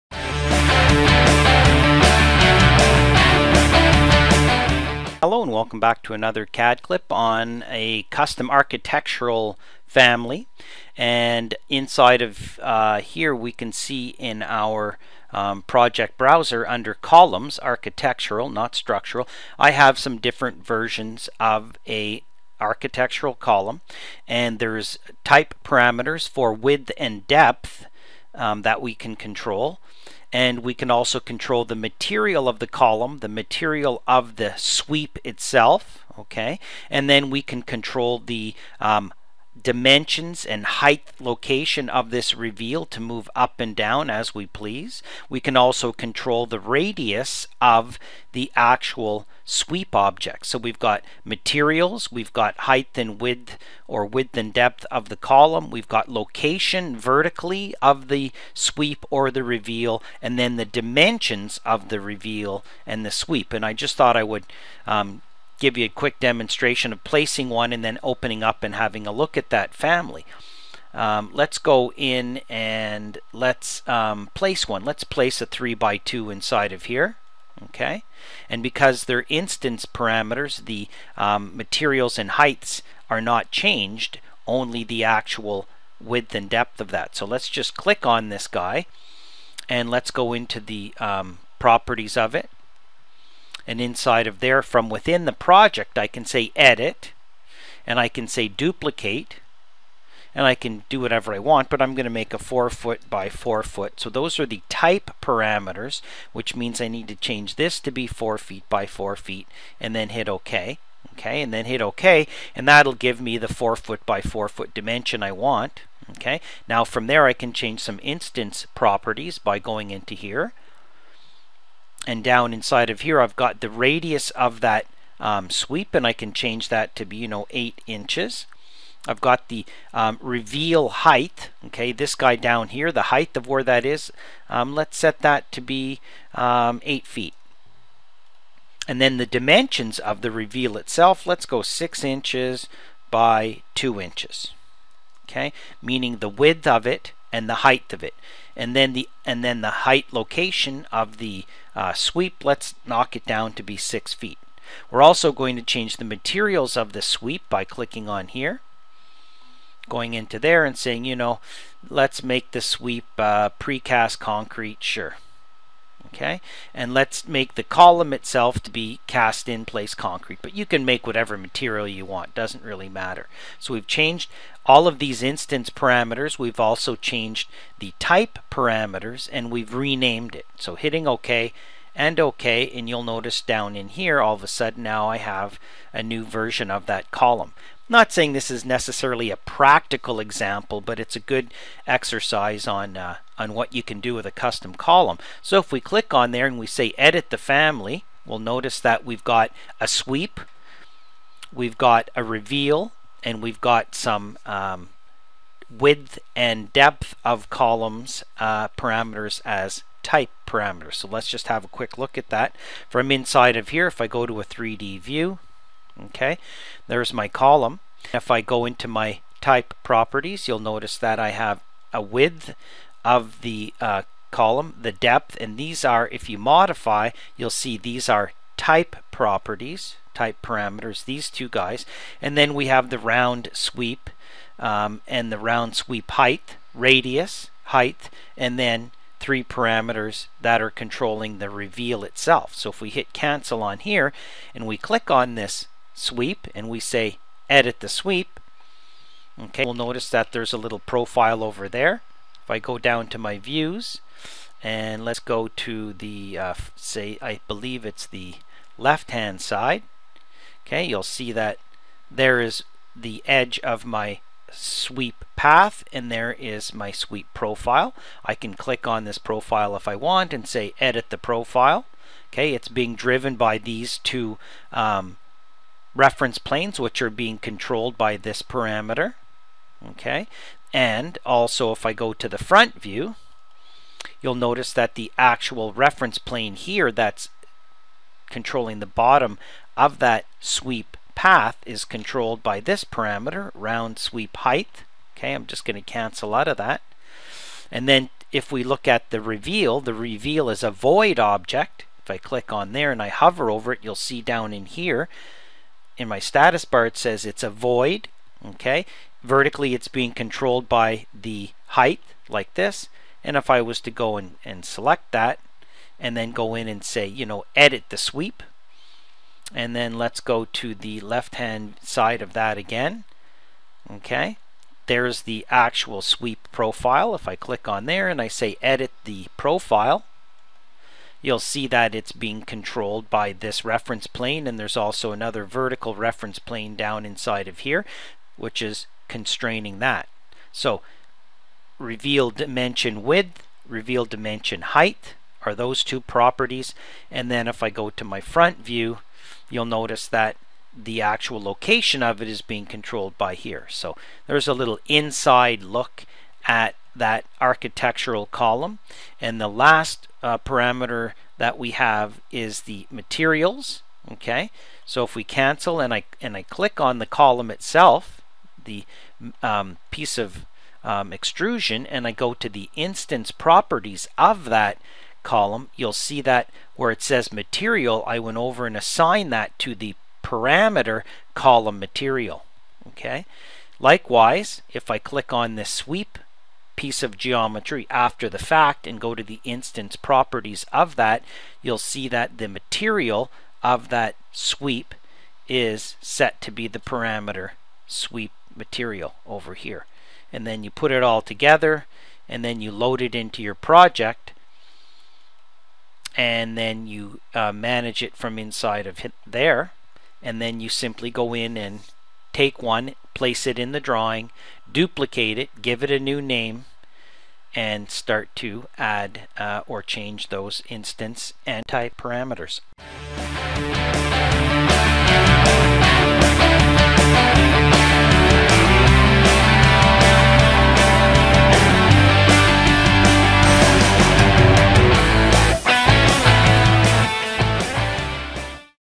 REVIT Custom Architectural Column Family with Sweep and Reveal - CADclip
This CADclips illustartes a custom REVIT architectural column family that has two type parameters for column width and depth and 7 instance parameters for column material, sweep material, sweep height, sweep radius, reveal height, reveal vertical dimension and reveal depth dimension.